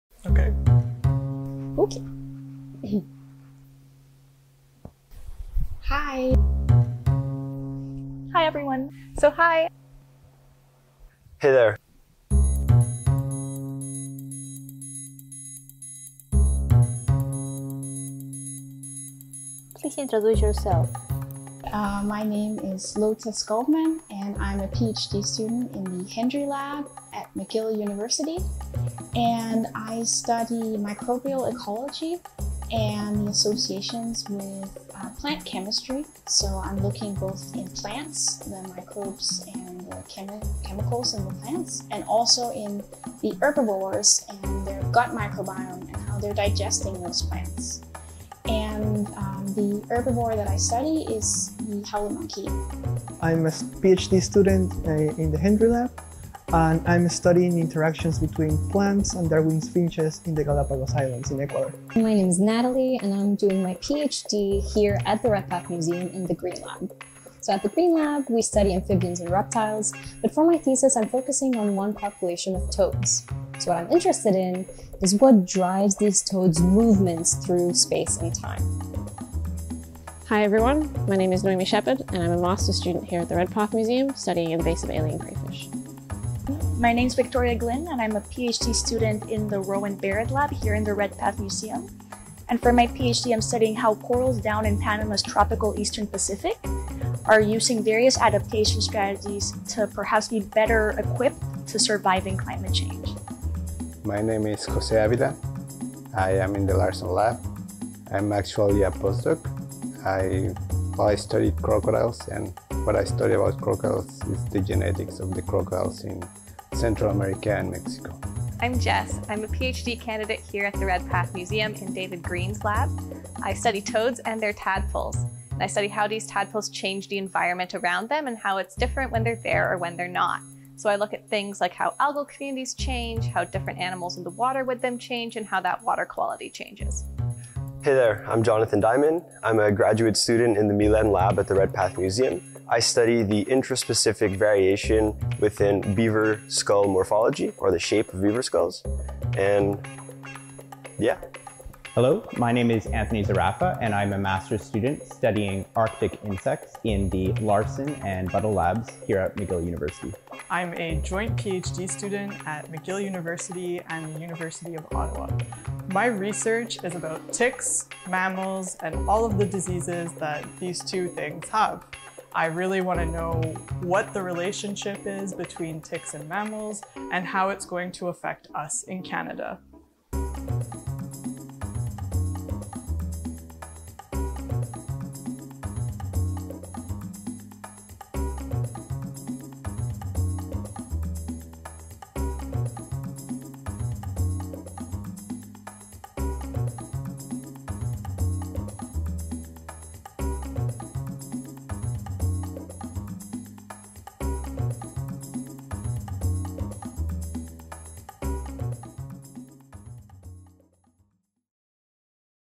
Capsules "Étudiants en vedette!"
Nous avons posé à 10 étudiants de cycle supérieurs du Musée Redpath de l’université McGill 3 questions sur leur carrière scientifique.
Dans cette première capsule, apprenez-en plus sur eux, avant de plonger dans les 3 questions!